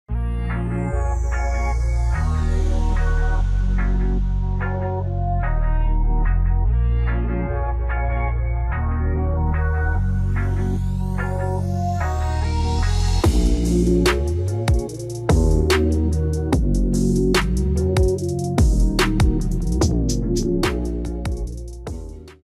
Murder Nova at the Quick Performance booth at PRI 2021
Walk around of Shawn Ellington's Murder Nova at the Quick Performance booth at the Performance Racing Industry in Indianapolis.